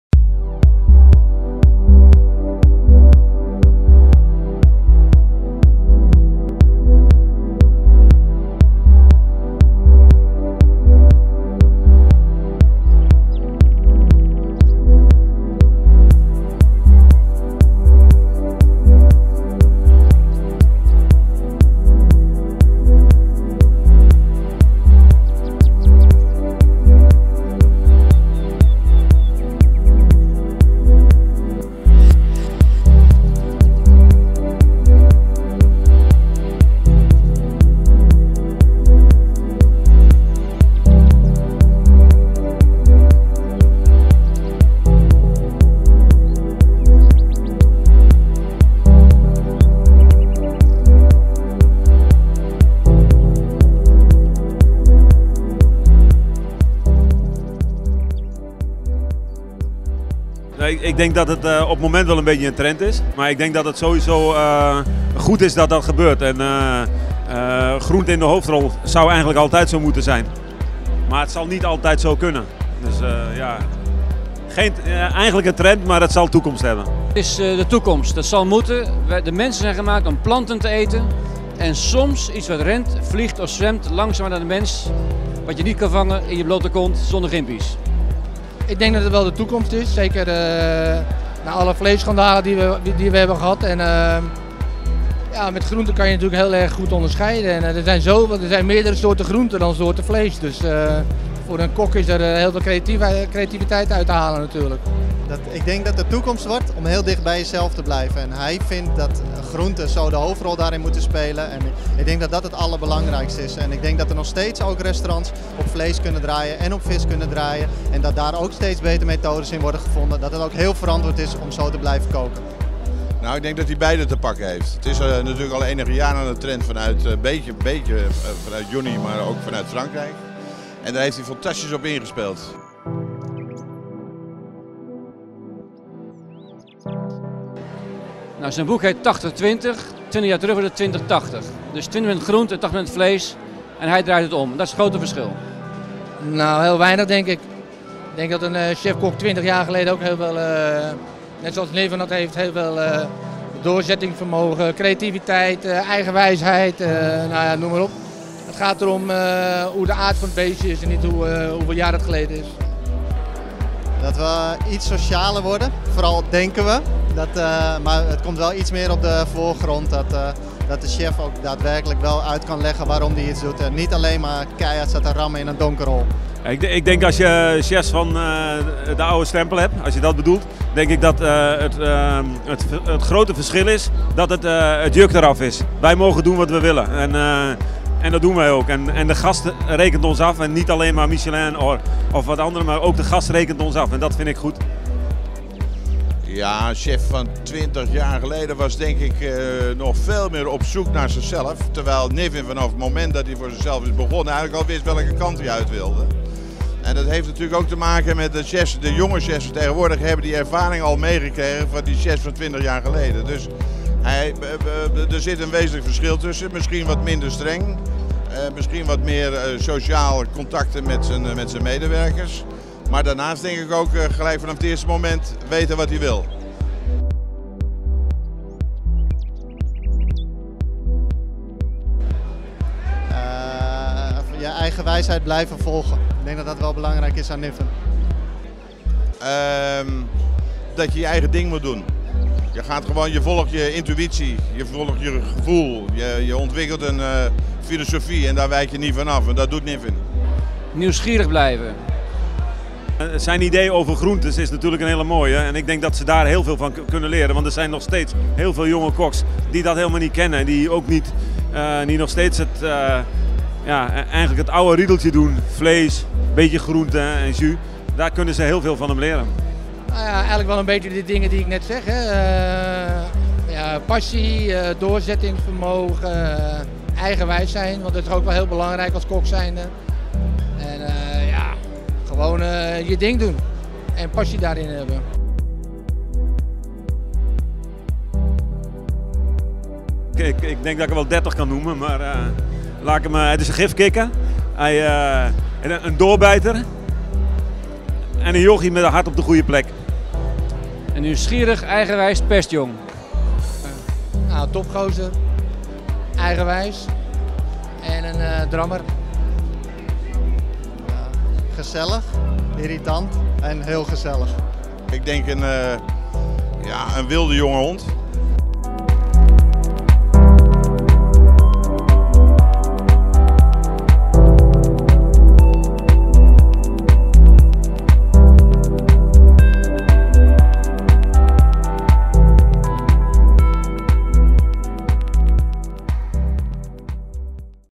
NIVEN8020 launch-party. club AIR, Amsterdam
Begin juni kwam NIVEN 80/20, het kookboek van Niven uit. De release was in AIR, de voormalige iT in Amsterdam. Een lancering van een kookboek in een nachtclub? Zeker!  Maar dan wel mét grasmat, boerenmarkt en een urban pick-nick voor een handje vol pers.

Er waren enorm veel collega's aanwezig waaronder oud leermeesters Lucas Rive en Jonnie Boer. Jonnie overhandigde samen met Thérèse het eerste exemplaar van NIVEN 80/20 aan Niven.